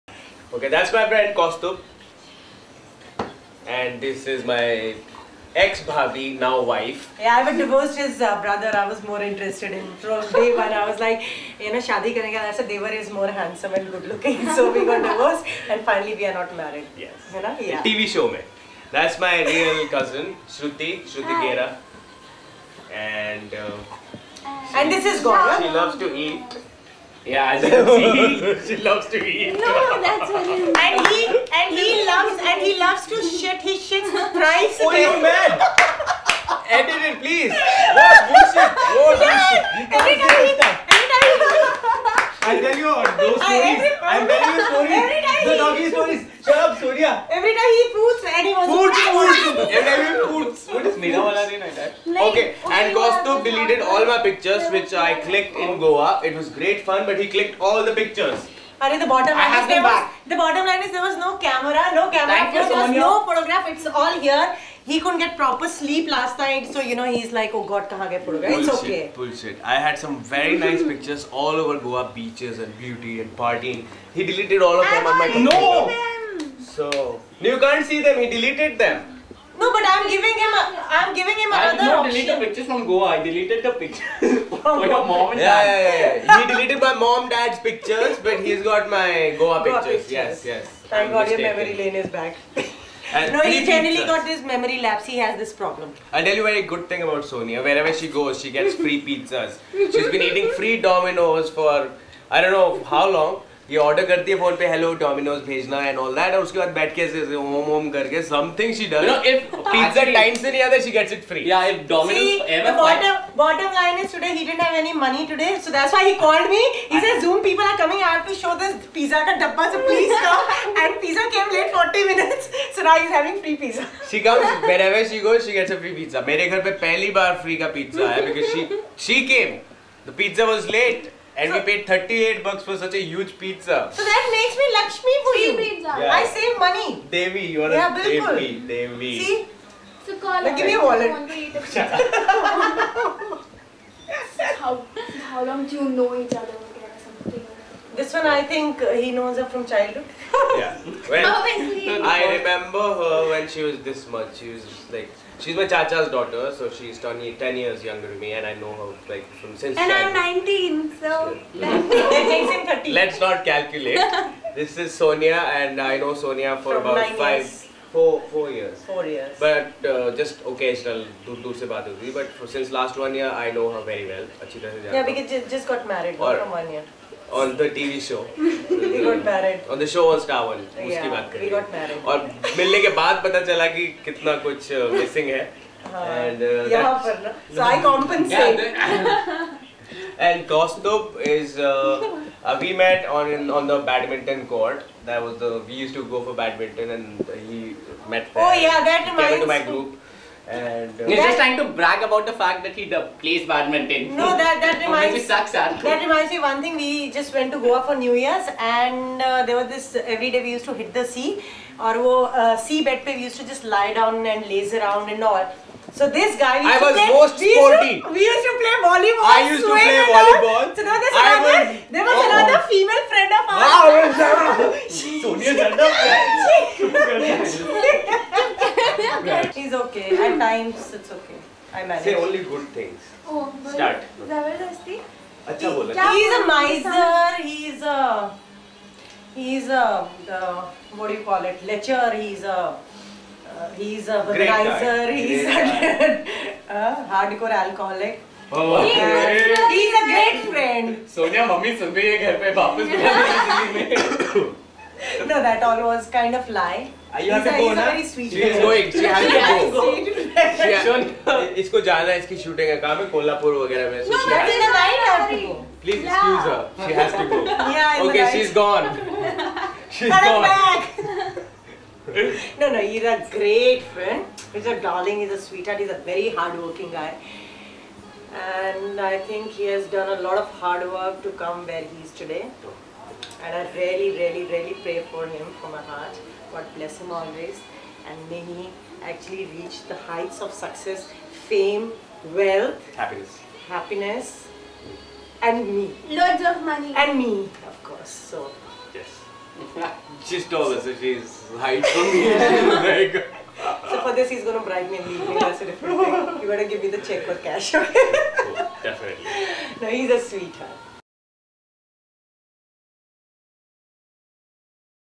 Gaurav Gera from Comedy Nights with Kapil with friends and family in Bombay apartment
A very young Gaurav Gera with his friends from the Tv industry and outside and family is filmed chilling with pizza at home in Mumbai.

Find out who his friends are and the funny banter between friends.